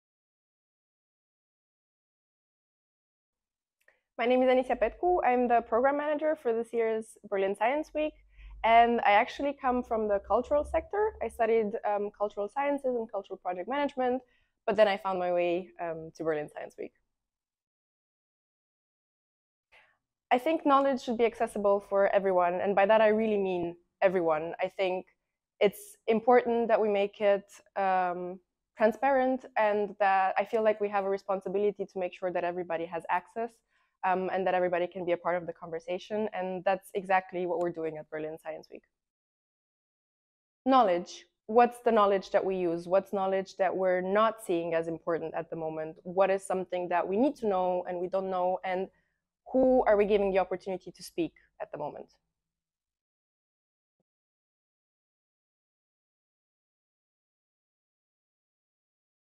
Anisia Petcu I Project Manager Programme, BERLIN SCIENCE WEEK
Anisia Petcu has a background in Literature and Cultural Science and has previously worked as a university lecturer and as a social worker dealing with issues of discrimination and racism. She wants to make knowledge and dialogue as open and accessible as possible, and to bring science to everyone, irrespective of background or social status. She is particularly excited about the interdisciplinary and internationality of the festival, which she sees as key towards inclusive and transparent scientific exchange and engagement.

In the Berlin Science Week team, she is responsible for coordinating and curating the programme - all 200 events of it!